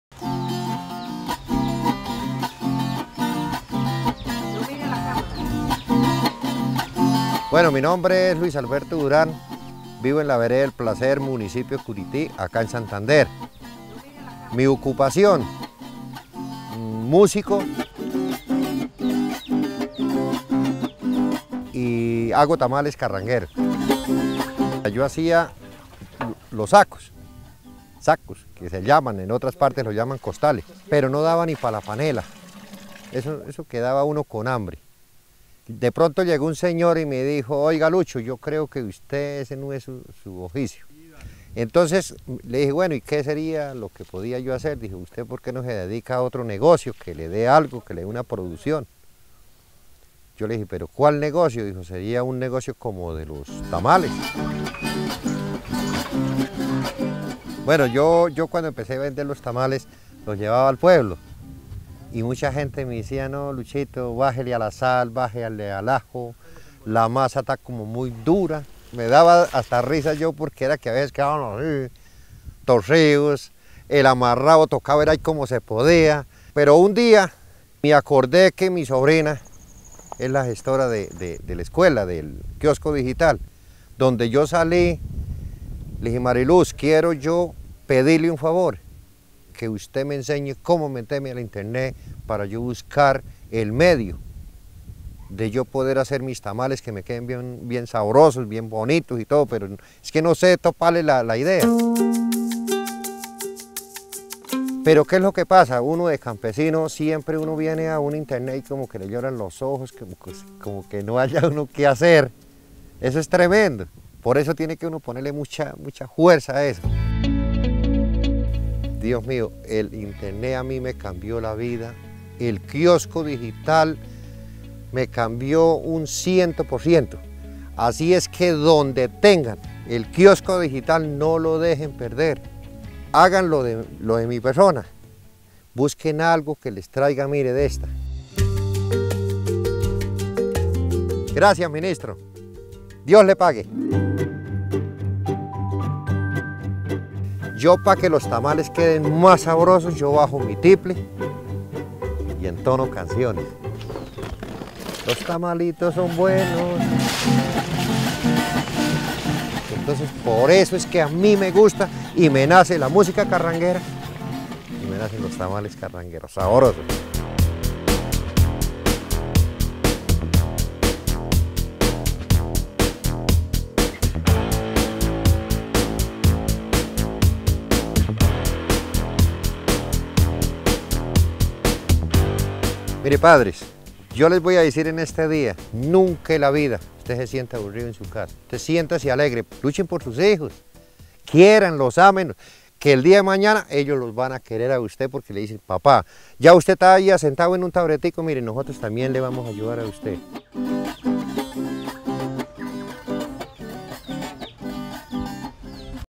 Kiosco ViveDigital C26 N6
Luis Alberto Durán, beneficiario Kiosco Vive Digital, se dedica a hacer tamales; aprendió a hacerlos, en el Kiosco, asegura que el Internet le cambió la vida.